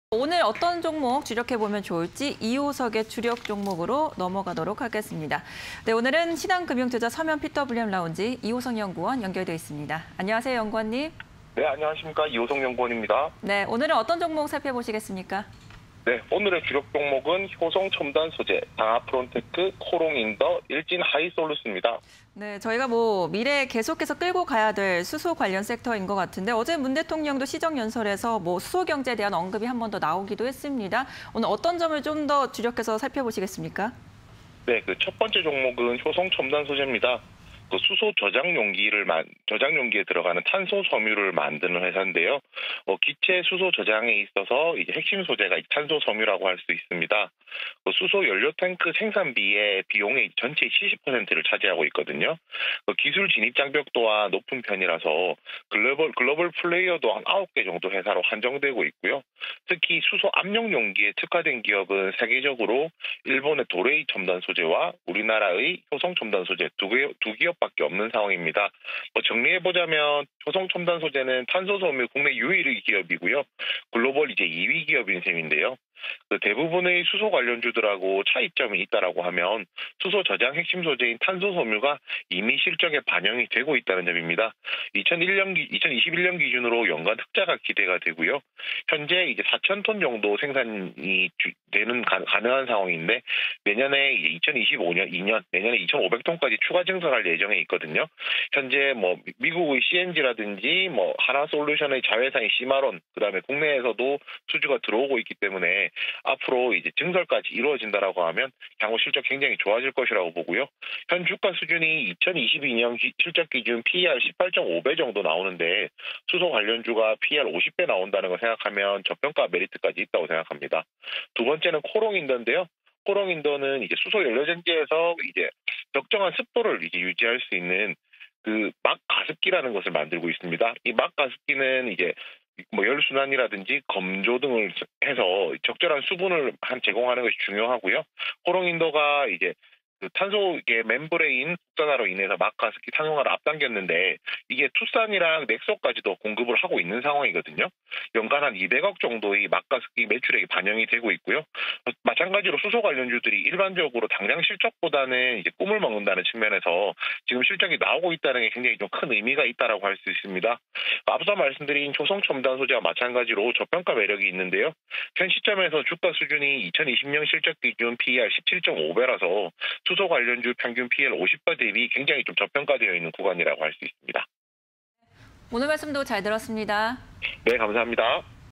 효성첨단소재, 코오롱인더, 상아프론테크, 일진하이솔루스_이호석의 주력 종목 (20211026)
1. 효성첨단소재(298050)
2. 상아프론테크(089980)
3. 코오롱인더(120110)
4. 일진하이솔루스(271940)

효성첨단소재(298050)
- 탄소섬유 제조: 기체 수소 저장 핵심소재
- 탄소섬유 실적 반영
- 2021년 기준 연간 흑자 예상
- 2022년 기준 PER 18.5배: 수소관련주 평균 50배 대비 저평가

코오롱인더(120110)
- 막가습기 생산: 수소 연료전지 습도조절에 중요한 역할
- 투싼·넥소 등 막가습기 공급
- 연간 200억 원 매출 반영
- 2022년 기준 PER 17.5배: 수소관련주 평균 50배 대비 저평가